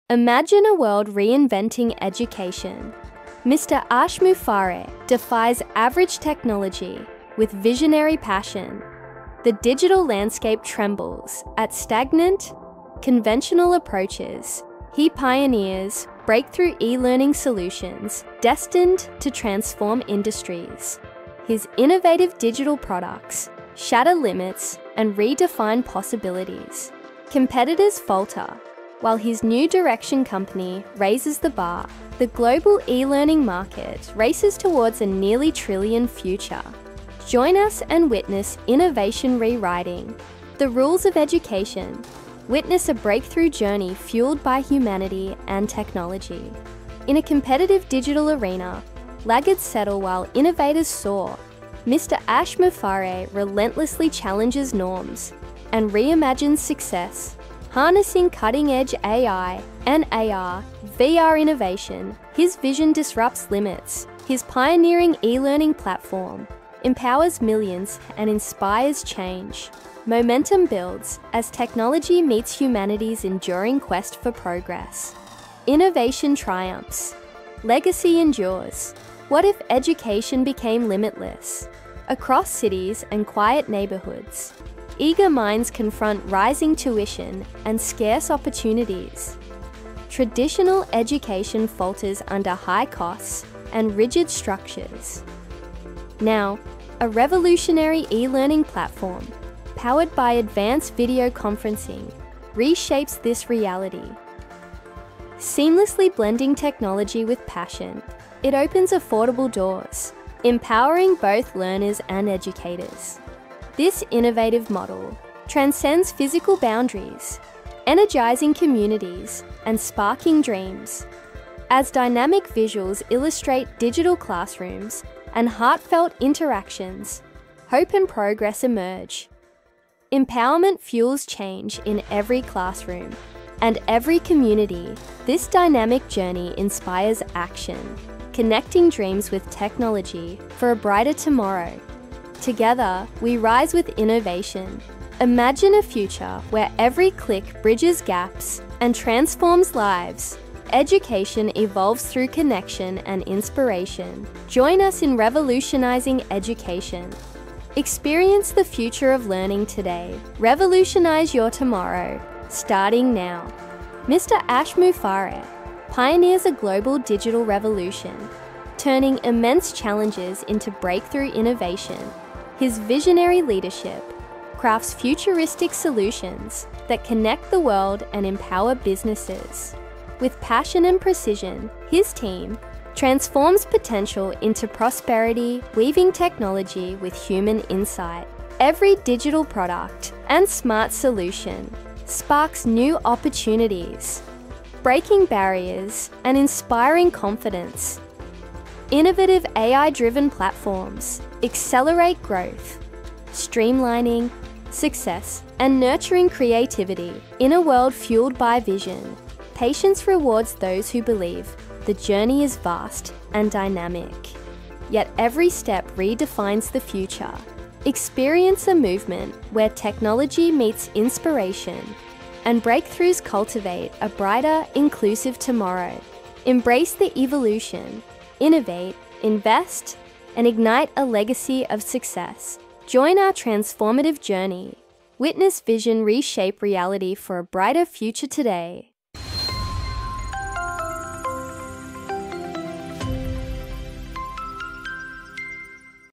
Ash Mufareh: Revolutionizing Education with AI The Trillion-Dollar Future of E-Learning
Witness the future of education unfold as visionary leader Ash Mufareh pioneers a global digital revolution. This is the story of how breakthrough e-learning solutions, powered by cutting-edge AI and immersive AR/VR technology, are shattering the limits of traditional education and creating a brighter, more accessible tomorrow.

In this video, we explore:
► Ash Mufareh's vision for a limitless, affordable, and empowering educational landscape.
► How innovative digital products and advanced video conferencing are replacing rigid, high-cost structures.
► The role of AI-driven platforms in accelerating growth and personalizing the learning journey.
► The global e-learning market's race towards a nearly trillion-dollar valuation and what it means for you.
► How technology, when fused with human passion, can connect dreams, bridge gaps, and transform millions of lives worldwide.

Join the movement that is rewriting the rules of education. This isn't just about new technology; it's about a fundamental shift in how we learn, grow, and succeed. Embrace the evolution and discover how to revolutionize your tomorrow, starting today.

Don't miss out on this fantastic opportunity! Register now and gain instant access to a distinguished range of free and paid products.